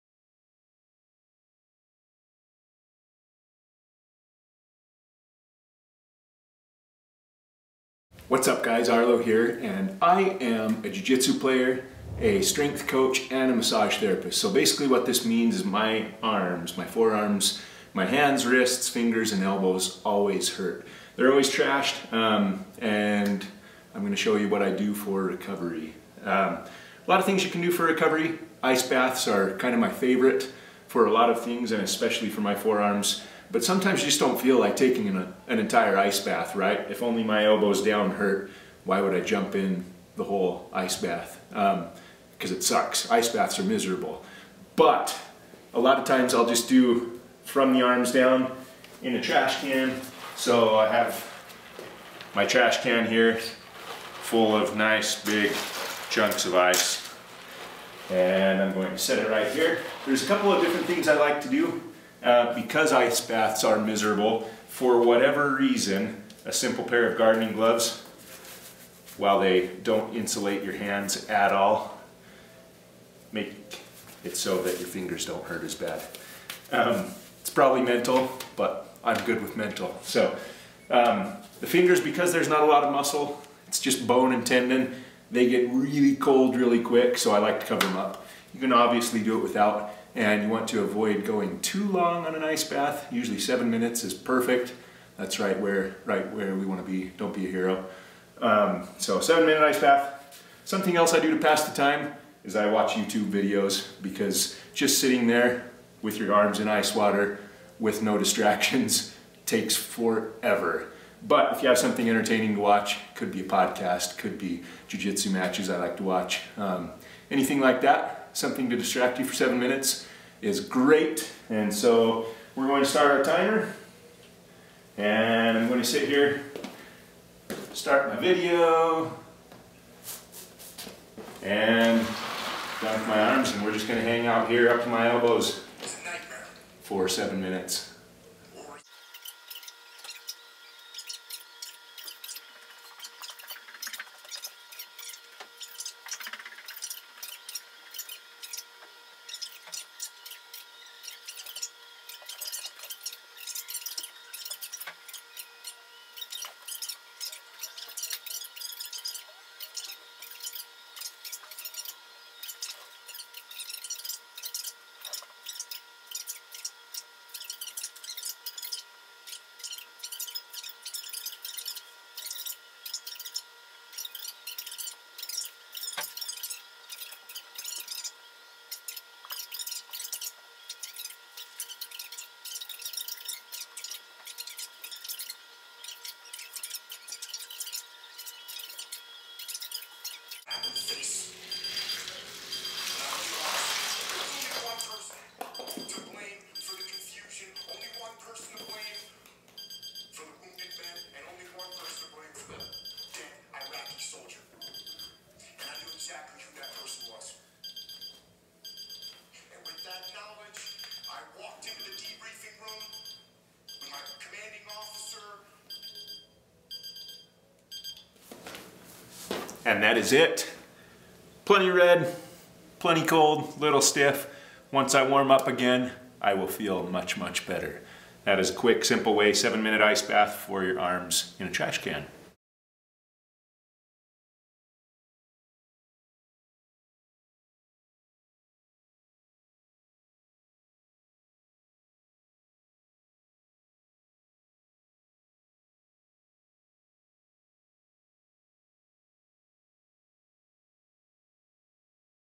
BT Recovery: Forearm Ice Bath
We grapple a lot. We also do farmer-walks, dead-lifts, pull-ups, and a lot of dedicated grip work. All of this volume can wreck your forearms. This is one of our favorite ways to address achy hands, wrists, and elbows. Stick to 7 minutes. When icing, more is not necessarily better.